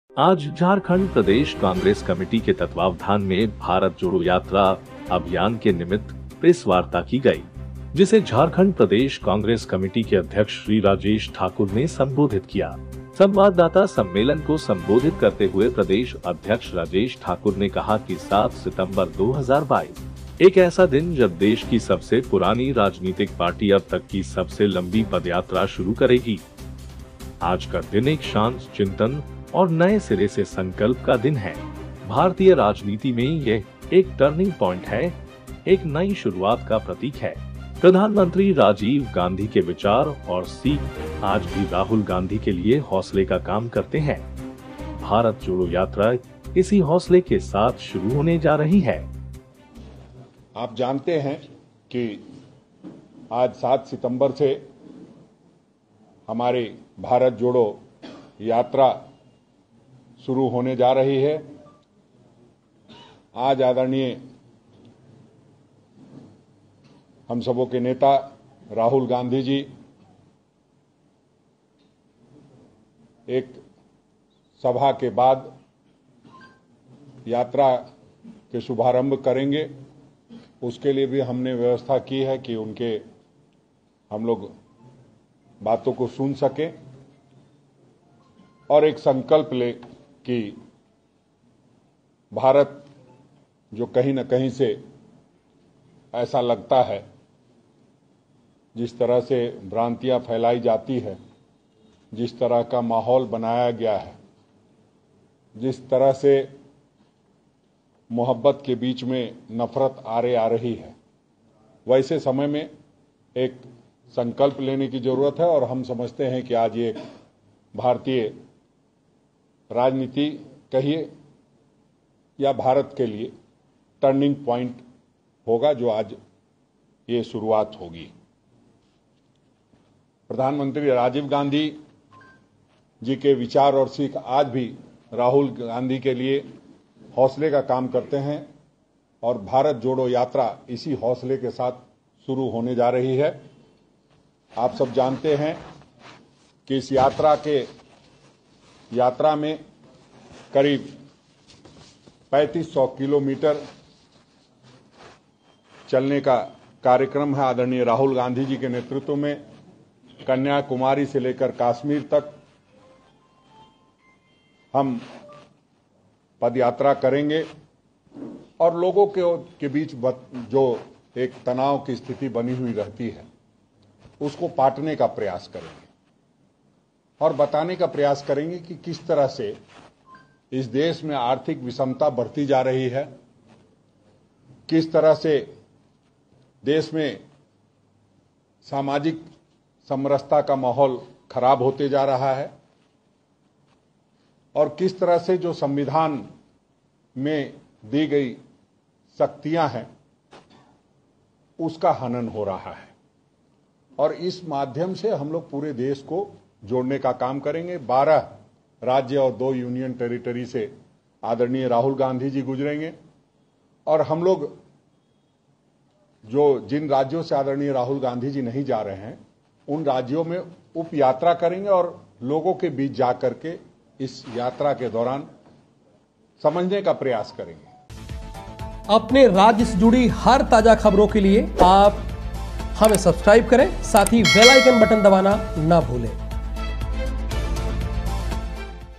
भारत जोड़ो यात्रा को लेकर हुई प्रेसवार्ता, सुनिए... प्रदेश अध्यक्ष Rajesh Thakur ने कह दिया बड़ी बात
भारत जोड़ो यात्रा को लेकर हुई प्रेसवार्ता, सुनिए... प्रदेश अध्यक्ष ने कह दिया बड़ी बात